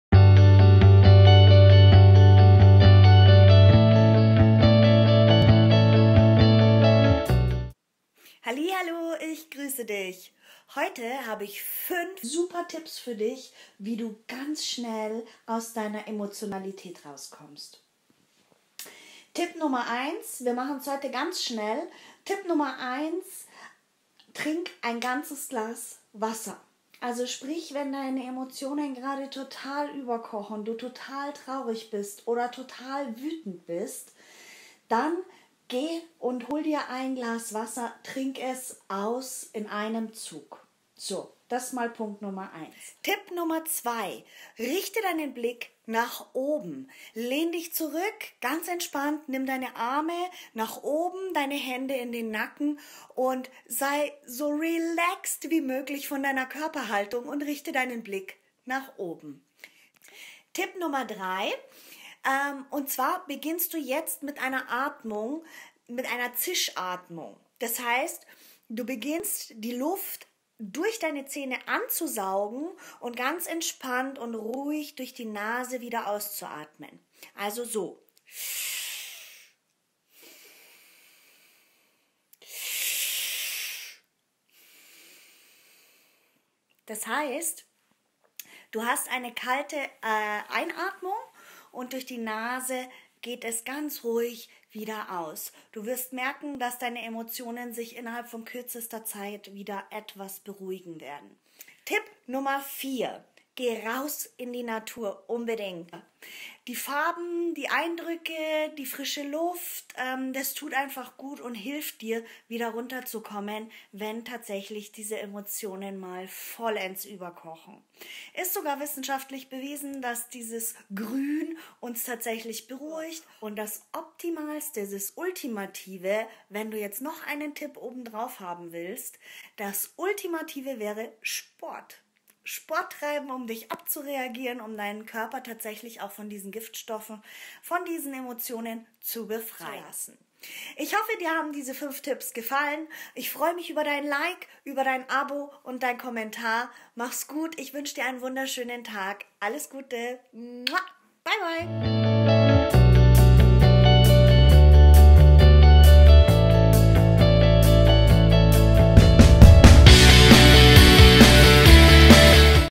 Wut stoppen, Traurigkeit überwinden. 5 Tipps wie du sofort aus deiner Emotionalität raus kommst!
kennst du das Gefühl von Wut, wenn die Hitze in dir aufsteigt und sie droht über zu kochen? Du hast das Gefühl du möchtest nur noch schreien?
Oder kennst du die Emotion Traurigkeit im falschen Moment? Wenn der Kloß im Hals immer drückender wird und jede Sekunde droht zu platzen? Und du den Tränen nahe bist?
Für solche Momente habe ich heute 5 geniale Tipps für dich, wie du innerhalb kürzester Zeit deine Emotionen beruhigen kannst!
Gib mir gerne Feedback, wie die Techniken bei dir funktioniert haben!
Teile gerne dieses Video und hilf damit auch deinen Mitmenschen, aus Emotionen auszusteigen und wieder mehr in die Balance zu kommen!
Ich freue mich sehr über dein Like! Danke das es dich gibt!

Ich wünsche dir alles Liebe, Licht, Kraft und Segen!